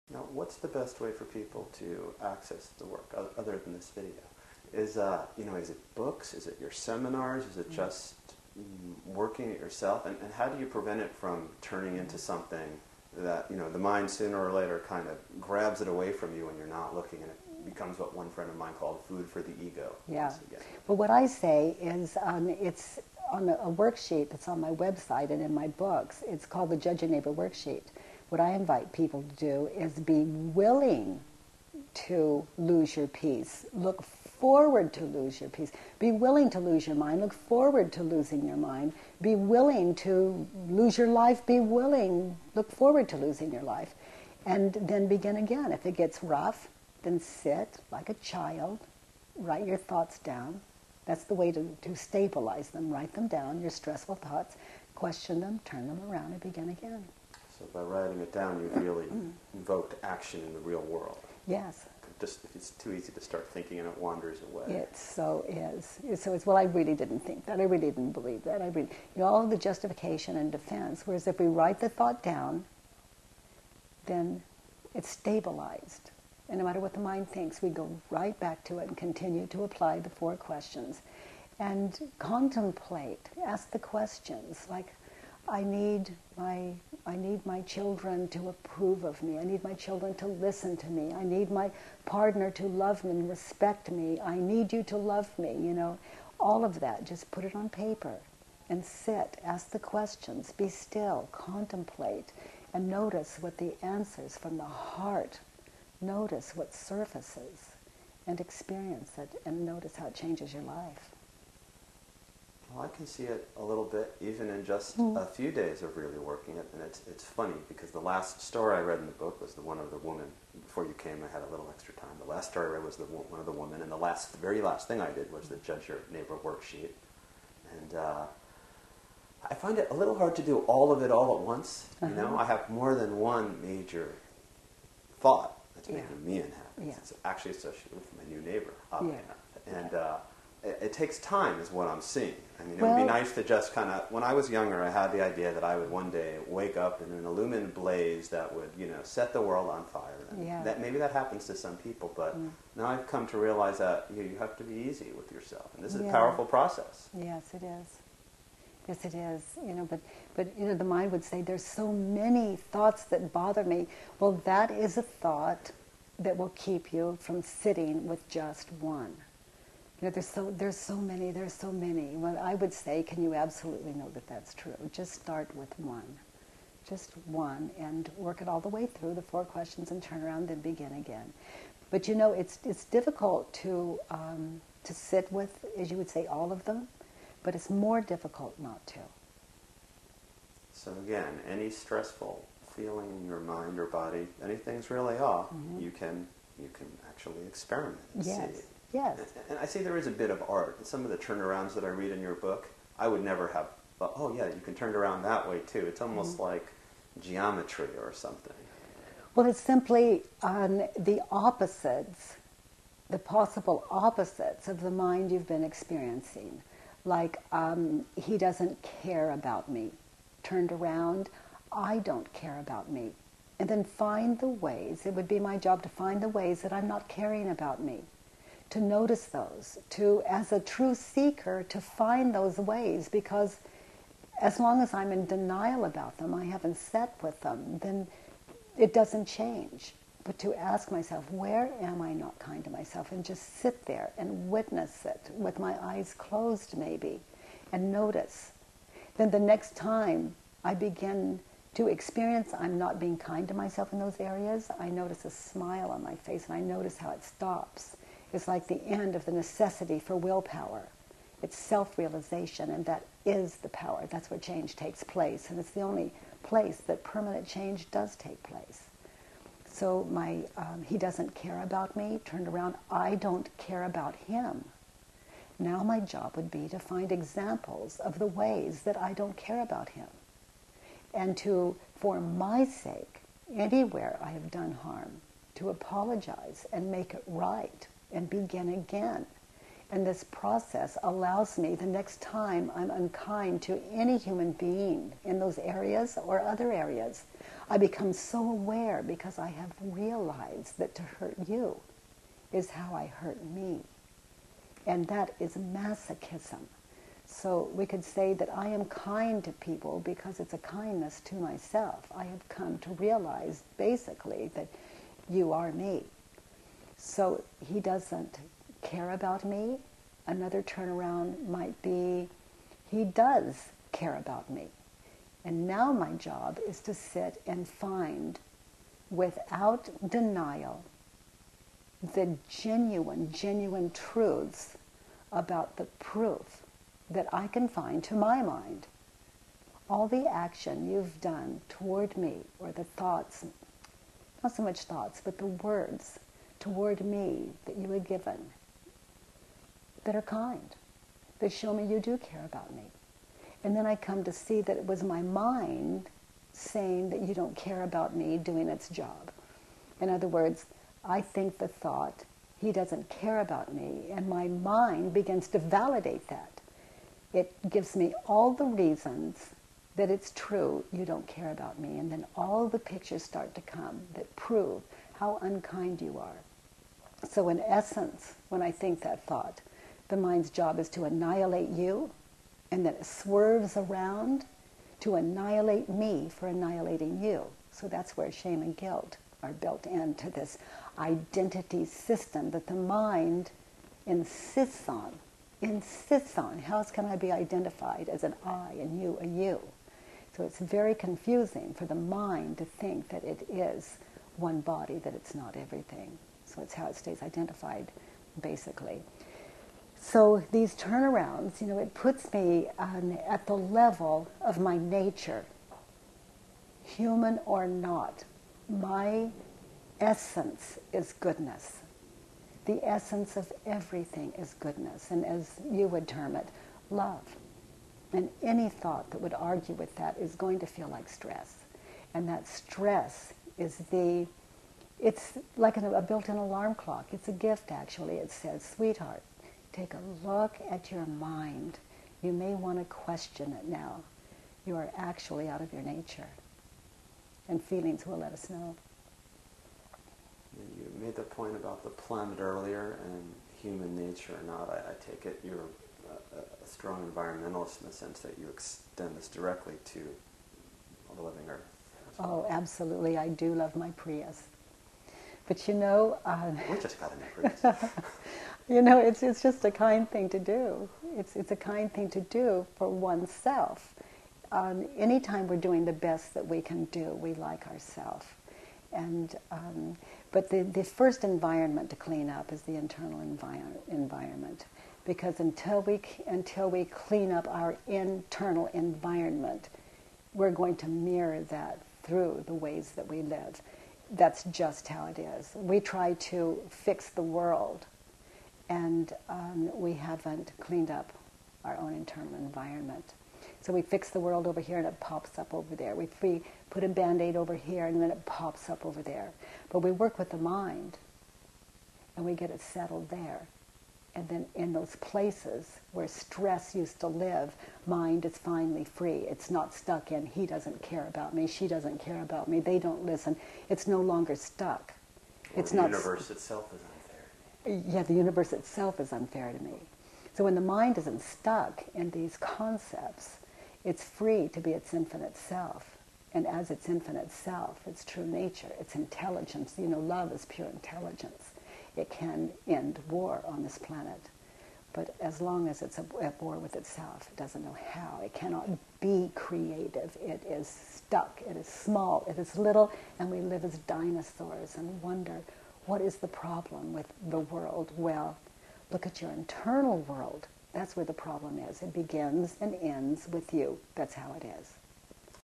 Byron Katie interviewed by EnlightenmentChannel.com in 2008 pt2
Byron Kathleen Mitchell, better known as Byron Katie (born December 6, 1942[1]), is an American speaker and author who teaches a method of self-inquiry known as "The Work of Byron Katie" or simply as "The Work".

The Work is a way of identifying and questioning the stressful thoughts that allegedly cause all the suffering in the world. It consists of four questions and the turnaround, which allows you to experience the opposite of what you believe.

The four questions asked of a stressful thought are: 1) Is it true?, 2) Can you absolutely know that it's true?, 3) How do you react, what happens, when you believe that thought?, and 4) Who would you be without that thought?